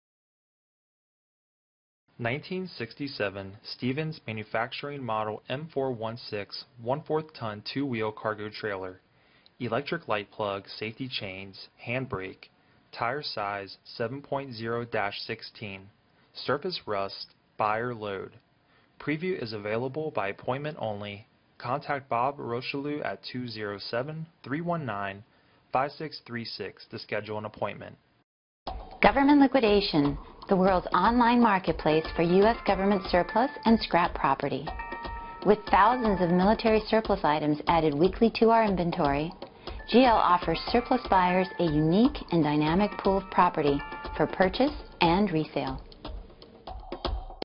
1967 Stevens M416 1/4 ton 2 Wheel Cargo Trailer on GovLiquidation.com
- This 1967 Stevens MFG Model M416 1/4 ton 2 Wheel Cargo Trailer has been sold.

This cargo trailer, located in Brunswick, ME, had an electric light plug, safety chains and a hand brake. The tire size was 7.0-16 and the trailer had some surface rust.

To view a current list of available military trailers, today!